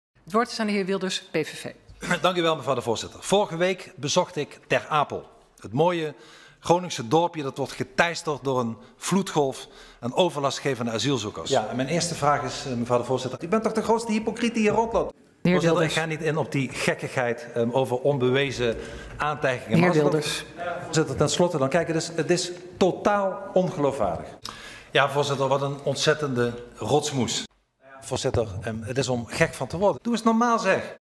Geert Wilders vs Weert Gilders
Geert Wilders heeft een debat met Weert Gilders in de Tweede Kamer. Geert Wilders sloopt de ideeën van Weert Gilders, terwijl hij gelijk probeert te krijgen.